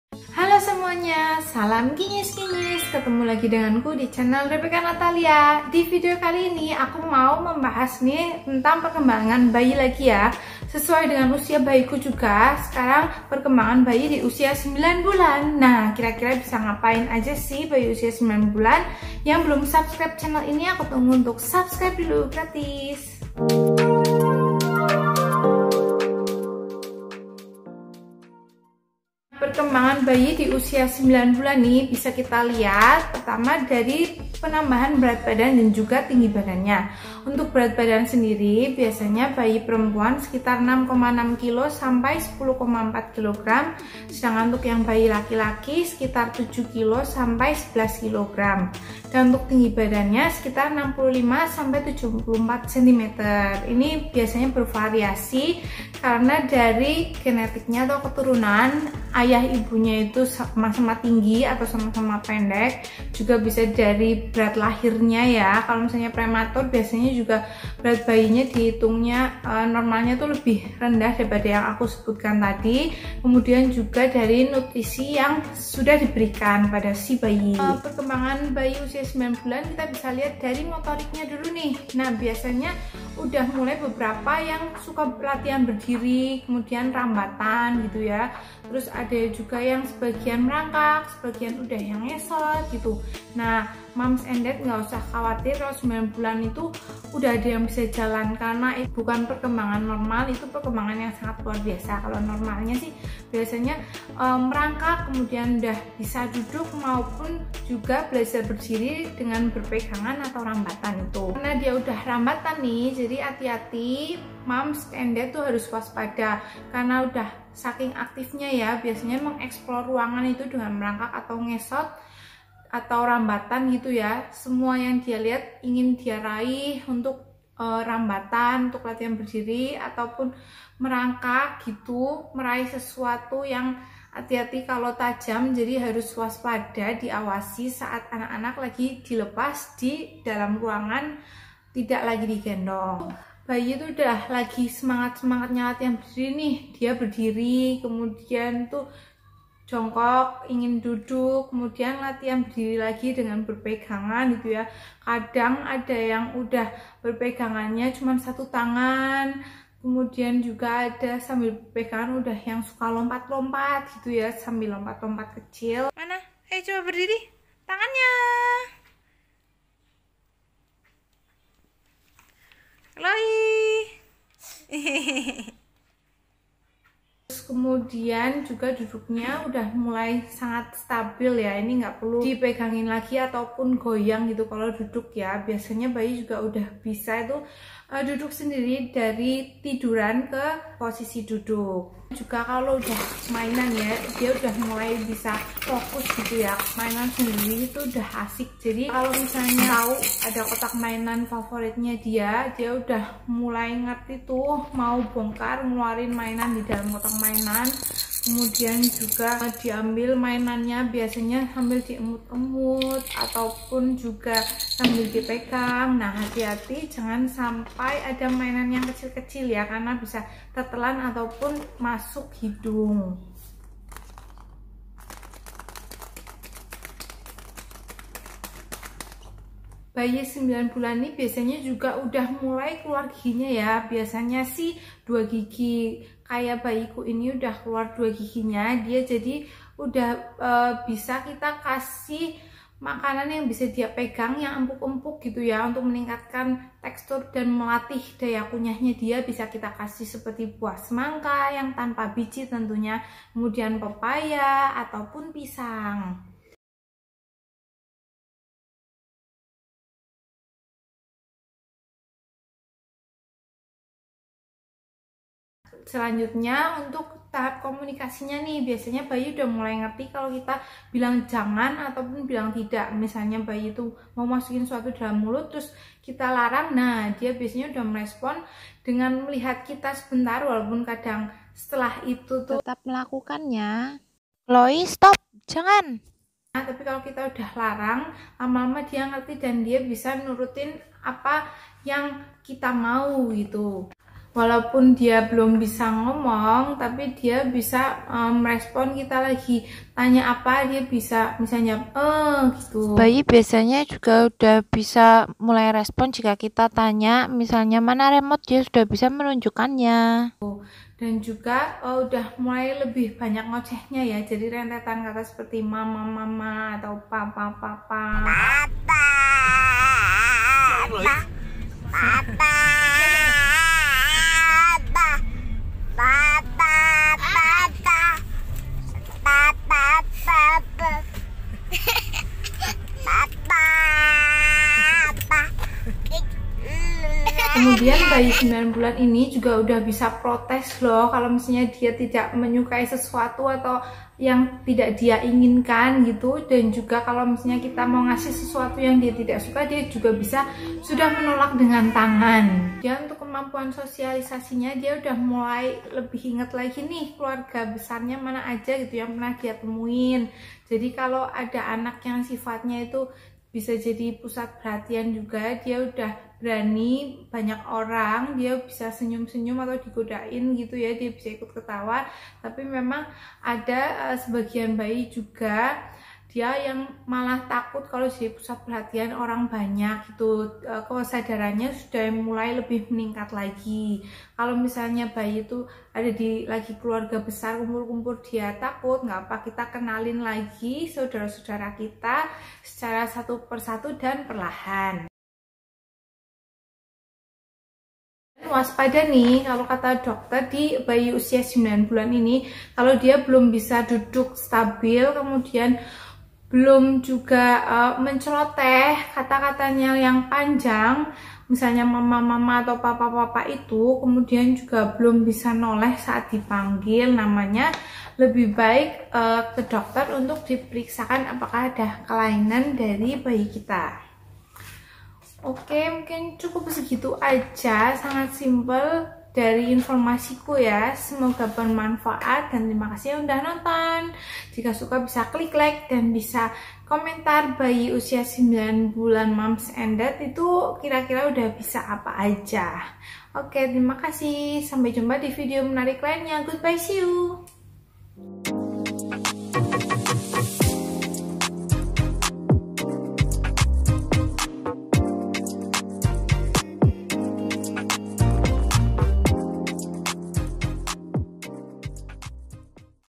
PERKEMBANGAN BAYI USIA 9 BULAN YANG MAKIN AKTIF
salamkinyiskinyis semua.. melanjutkan tentang perkembanganbayi yang sekarang usia9bulan bisangapainaja ya.. pastinya bayilucu makinaktif juga..
liat selengkapnya divideo ini ya.. happy watching!😻